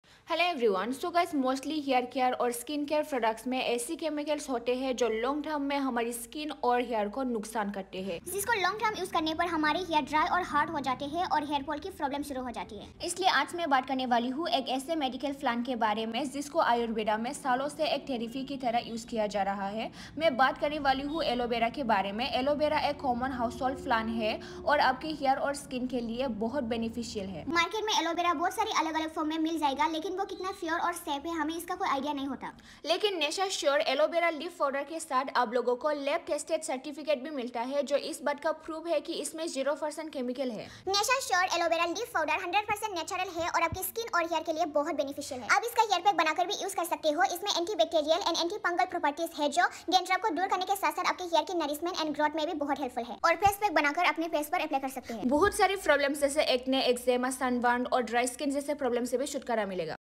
Soft & Glowing Skincare at home || Hair & Face Mask || All Skin Types || Jenia Brahma
Natural solution packed with powerful nutrients that can help to nourish and protect your skin & hair from damage, and helps you to achieve flawless skin and gorgeous hair.

Nature sure Aloe vera Leaf Powder